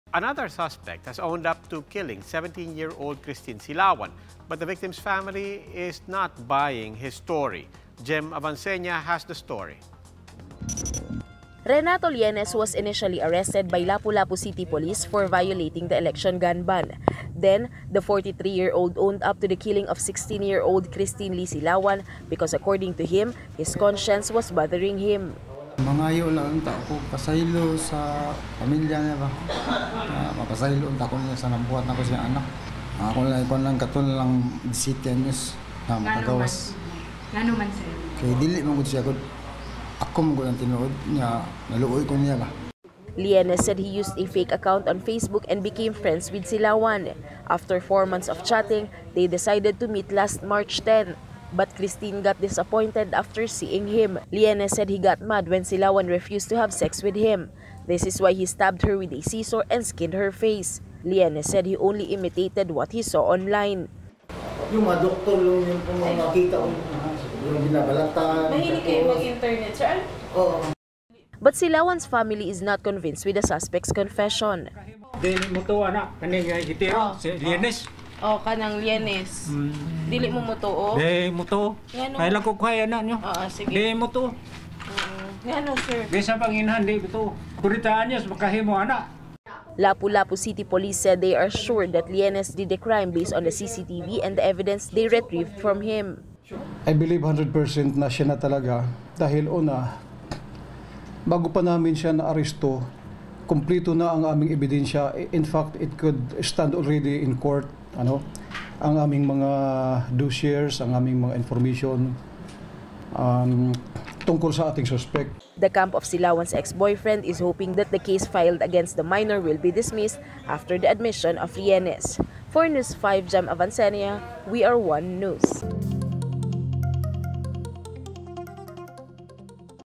Suspect in Christine Silawan slay case admits crime
The family of slain Cebu teen Christine Silawan is not buying the story of the second suspect who owned up to the 16-year-old's murder.